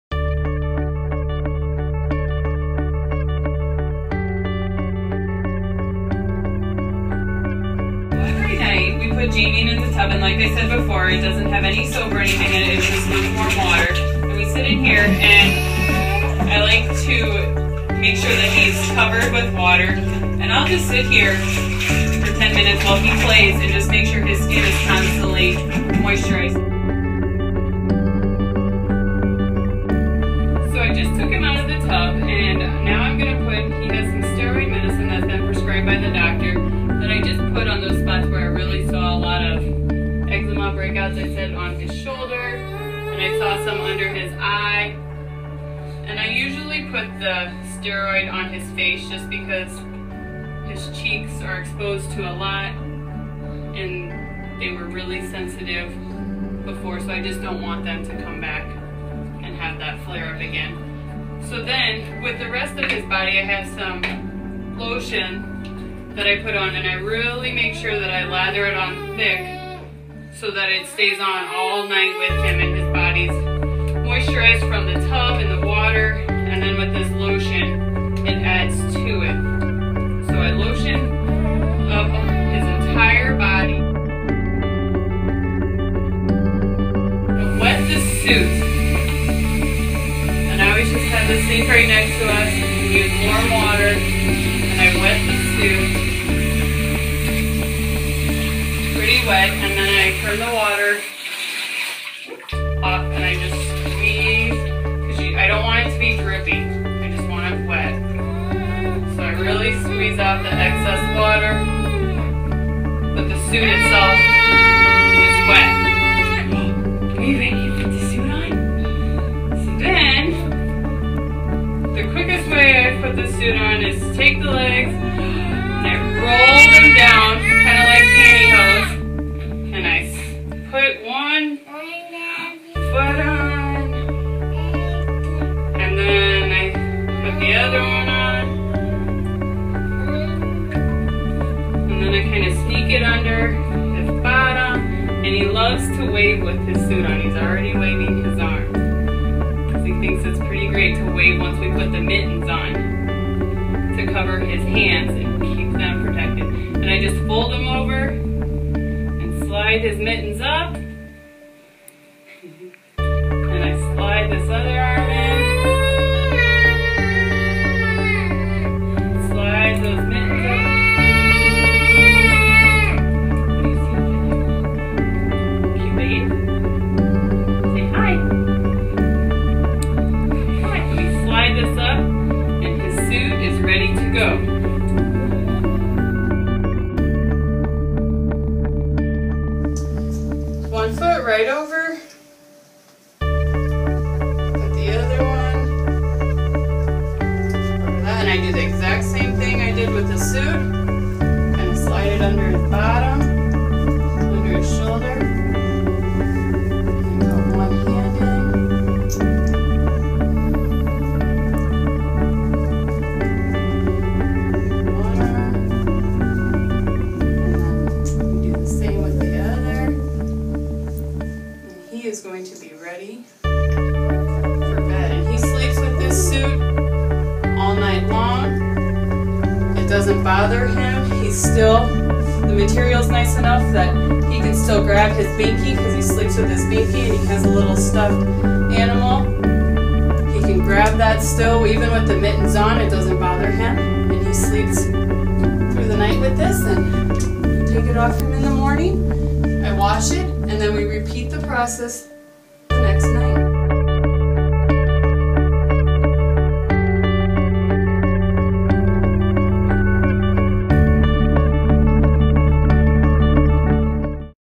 The AD RescueWear Wet Wrap Therapy Demonstration
Does your child suffer from eczema?  Wet Wrap Therapy is the Relief You'e Been Waiting For.  Our Wrap-E-Soothe products make wet wrap therapy quick, easy and effective.